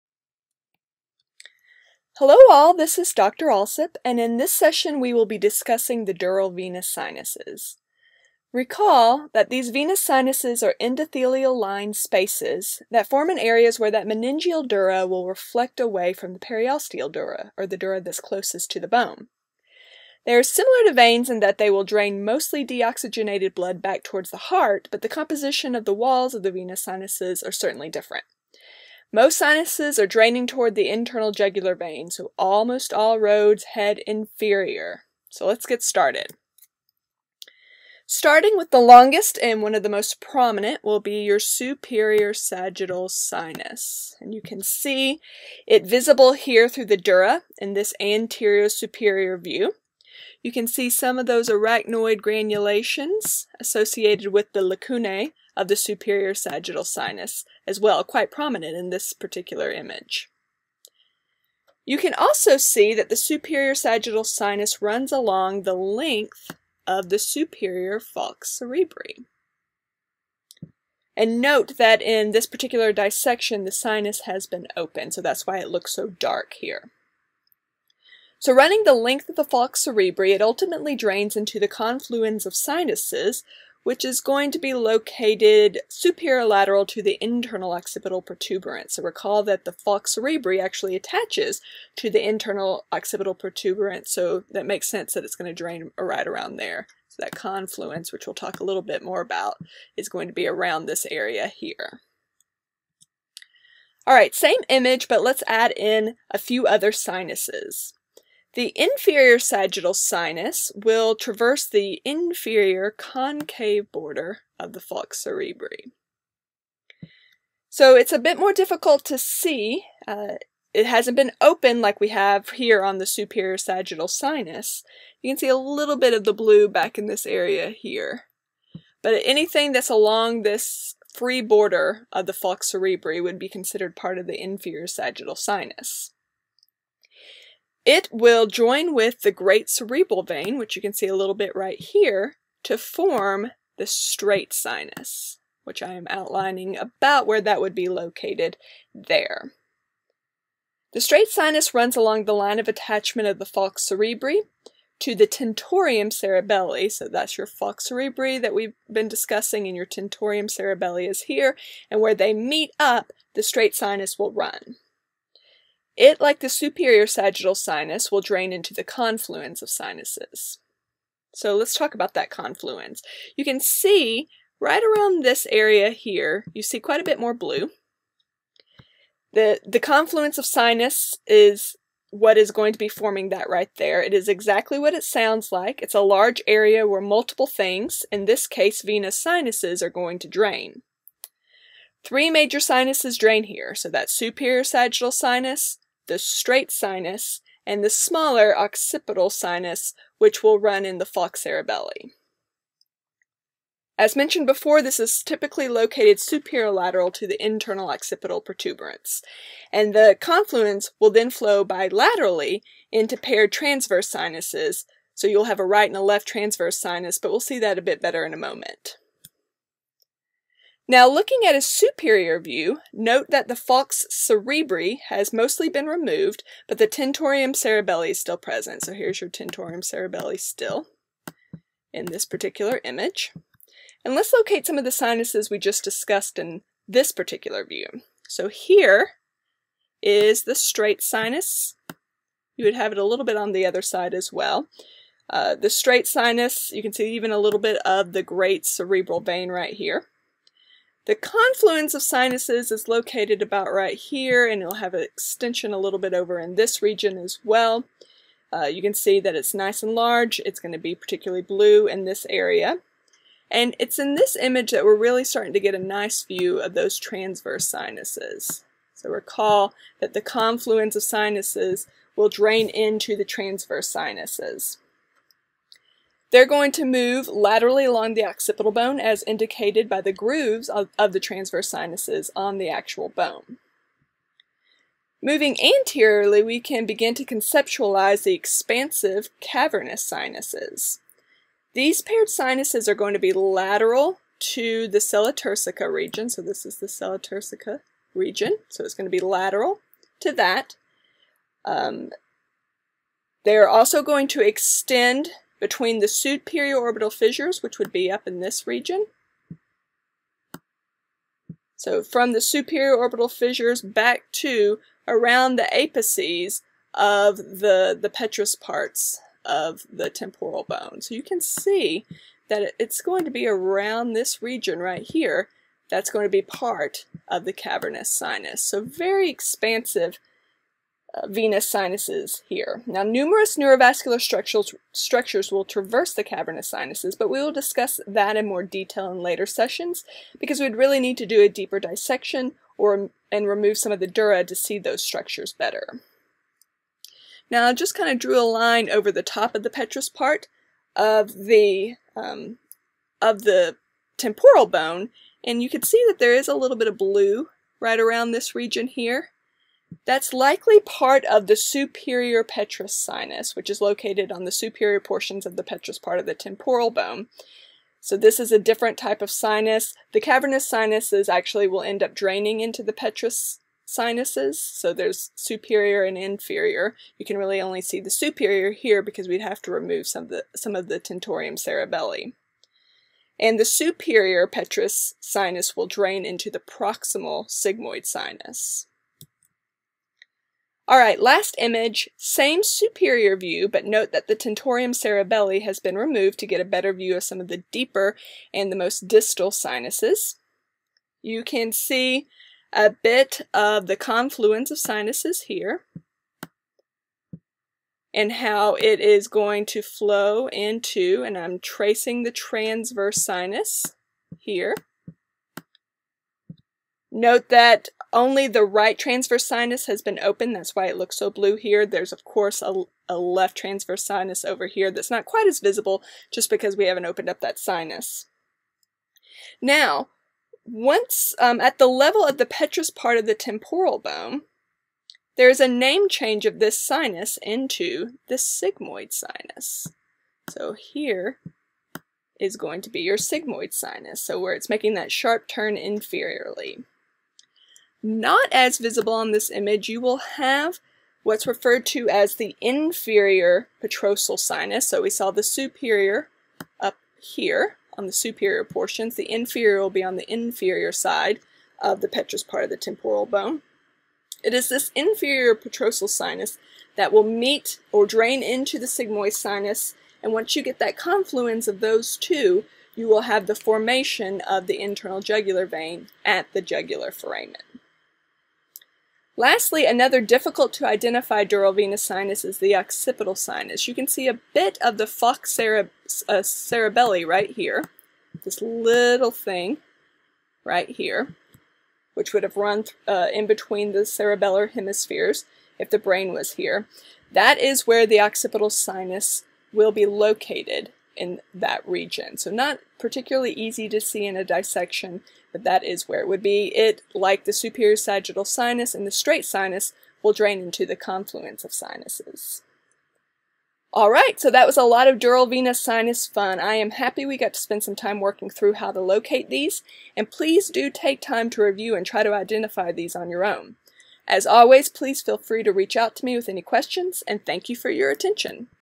Cranial Cavity and Brain - Dural Venous Sinuses
Video describing tips to best identify the dural venous sinuses associated with the cranial cavity and brain in relation to the supplemental dissection videos for the DENT 545 Dental Head and Neck Anatomy course at the University of Michigan.